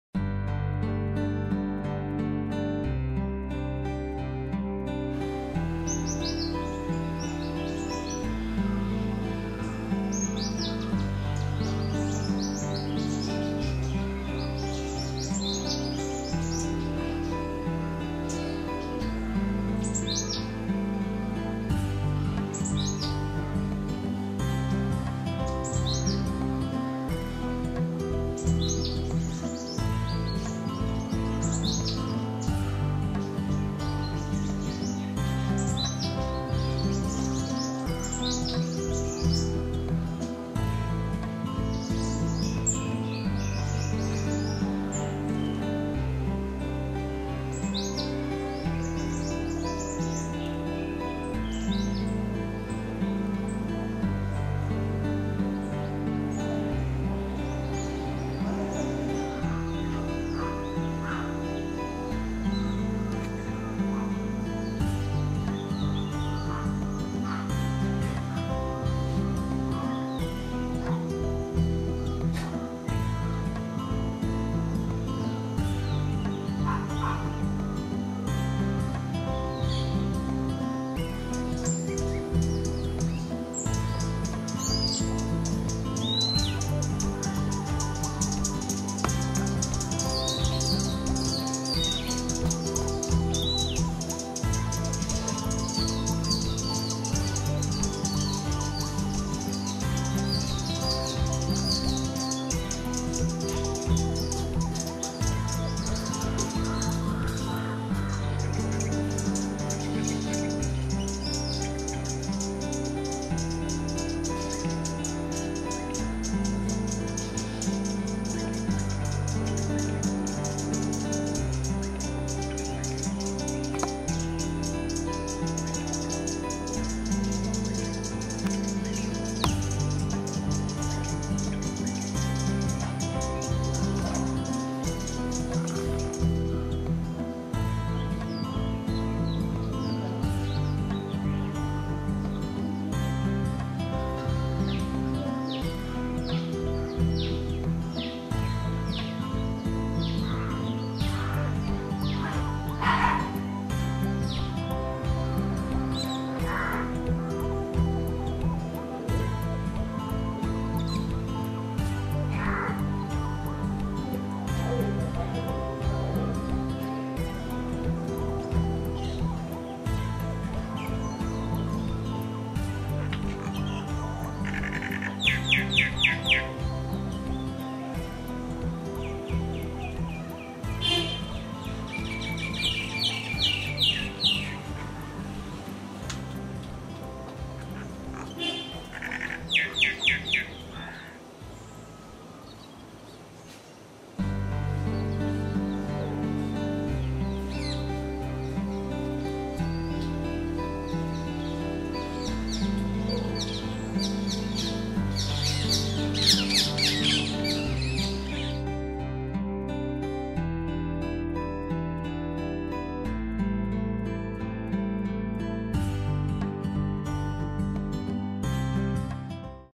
Birds of Sri Lanka (Ceylon) - Sri Lanka Tourism : Visit Sri Lanka 2011
This video is a collection of clips of birds captured in my backyard. The video location is about 1.5 km away from the Kandy town centre. This aim of this video is to entice Birdwatchers and Eco-Tourist from around the world to visit Sri Lanka. The clips were shot over a period of few hours, which is proof of the abundance of unspoilt nature in the little Island of Sri Lanka.

The video features the Scarlet (Orange) Minivet, Red Vented Bulbul, Purple Rumped Sunbird, Oriental White Eye, Greater Coucal, Yellow Billed Babbler, Indian (Common) Myna and Rose Ringed Parakeet.

Tourism is vital to the economy of Sri Lanka, specially since it is recovering from over three decades of war. Tourism would help to strengthen the Nations economy to usher in an era of Peace and Stability to small Island nation of very friendly, hospitable and smiling people. Sri Lanka has something to offer every Traveller and is undoubtedly "A Paradise of Diversity and Wonders". Year 2011 is the "Visit Sri Lanka Year".

Sri Lanka welcomes you all to an incredible experience!

HD Video available on CC Non-Commercial Attributed